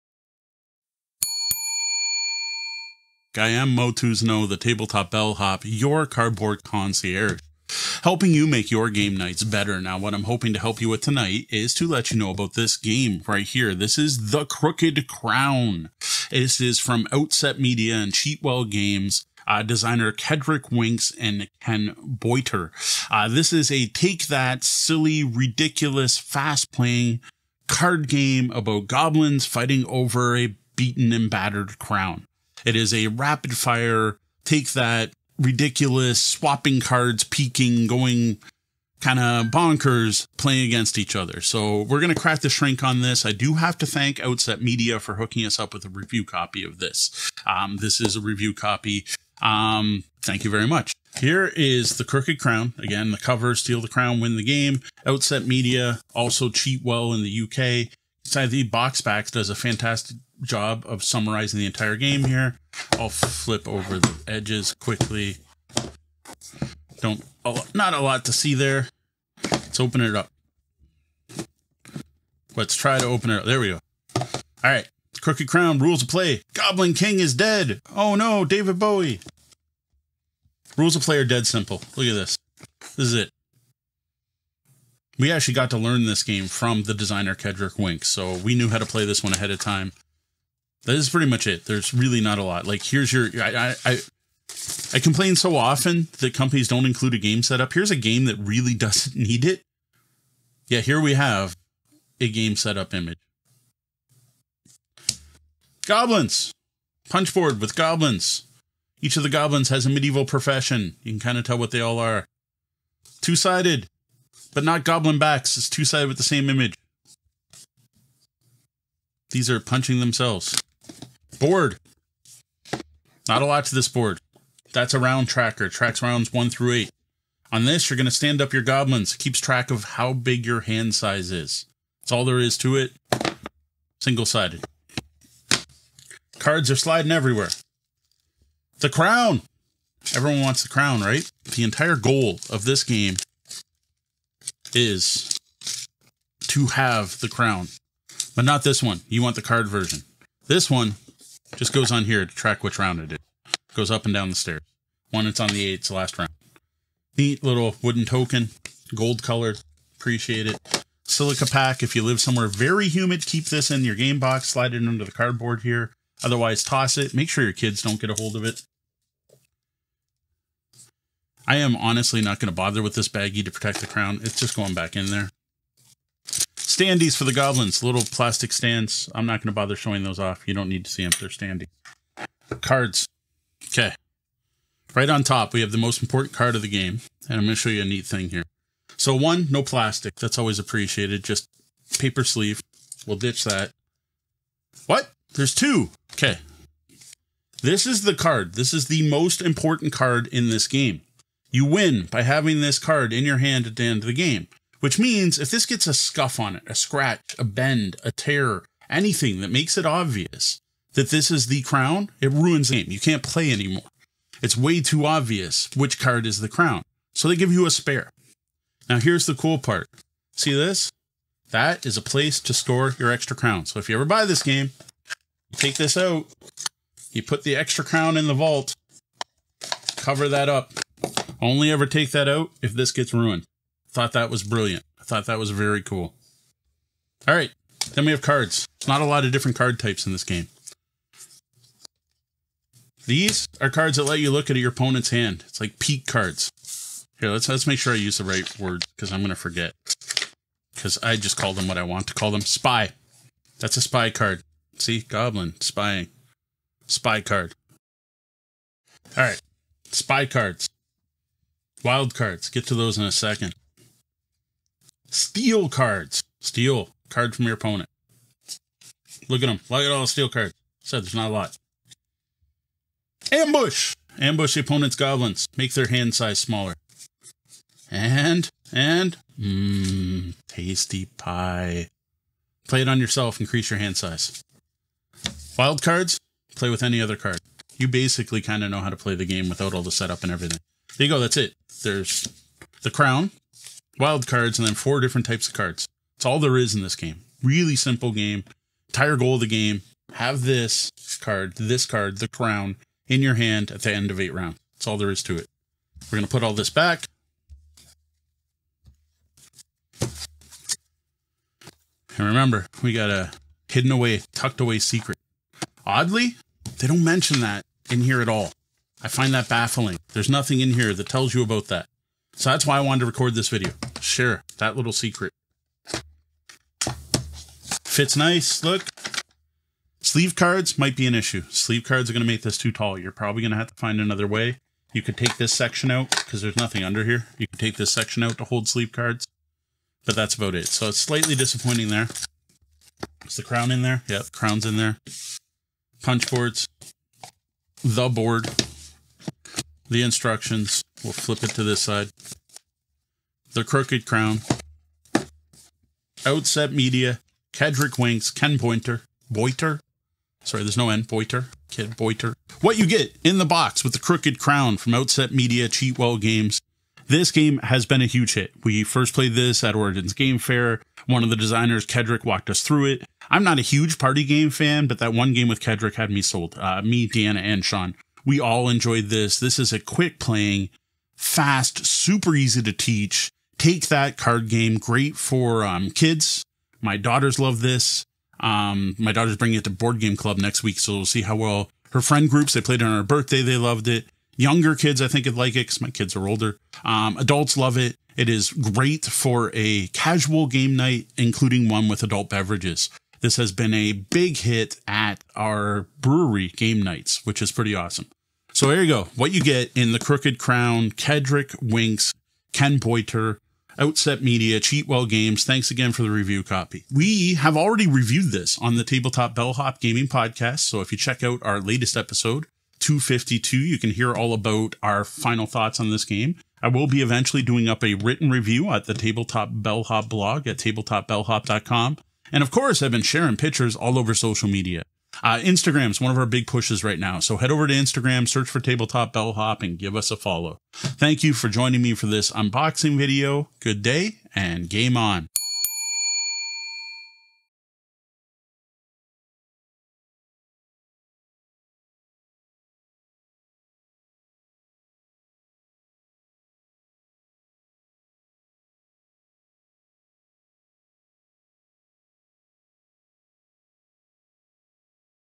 The Crooked Crown Unboxing, A take-that, backstabby, board game about goblins fighting over a crown
Steal the Crown, Win the Game! Take a look at what you get with the Crooked Crown board game.  Spy on each other, manipulate hand sizes, and steal cards, to make sure you are the one holding the crown at the end of eight rounds.

Index:
00:00 INTRO
00:50 OPENING UP THE CROOKED CROWN FOR THE FIRST TIME
06:02 THE CROOKED CROWN HAS BEEN A BIG HIT
06:56 CLOSING AND WHERE TO FIND MORE

The Crooked Crown is a game of bluffing, tension, and treachery! Use your cards to commit dastardly deeds with the goal of claiming the crown! But once you have it, you have to hold on to it! So you'll need to cast suspicion on others players and bluff your way to victory!

A game by Kedric Winks and Ken Boyter.
Includes: 78 cards, 6 goblin tokens, throne room playing board, wooden crooked crown token, instructions